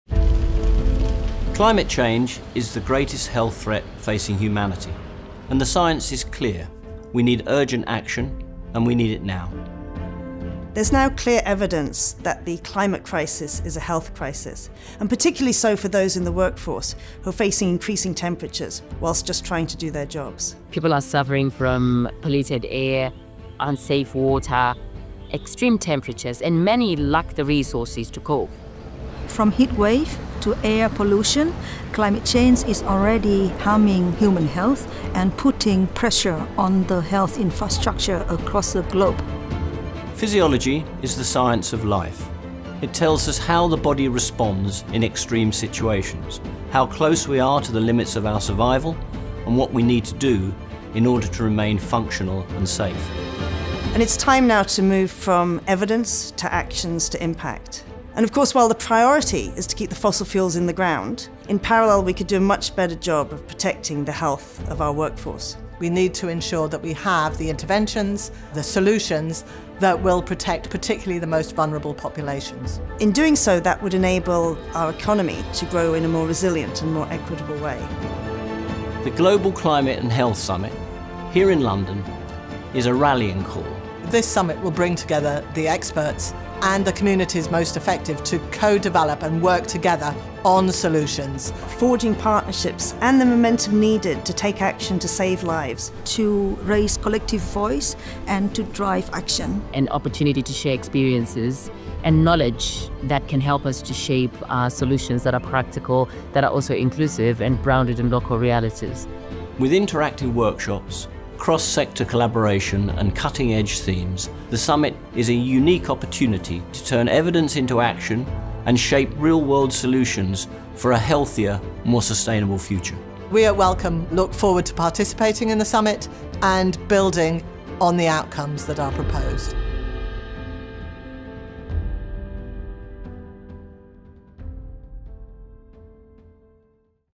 Global Climate and Health Summit | Addressing the Intersection of Climate Change and Human Health
Climate change is the greatest health threat facing humanity—and the science is clear: we need urgent action, and we need it now.

In this video, hear from the leadership of the Global Climate and Health Summit 2025, including Chair Professor Mike Tipton, Vice Chairs Dr. Fortunate Machingura and Ahn Vu, keynote speaker Professor Elizabeth Robinson, and Madeline Thomas from Wellcome.

Together, they highlight the urgent need to move from evidence to action, protect vulnerable communities, and build a healthier, more resilient future.

🌍 Join us in London for the Global Climate and Health Summit. globalclimateandhealthsummit.org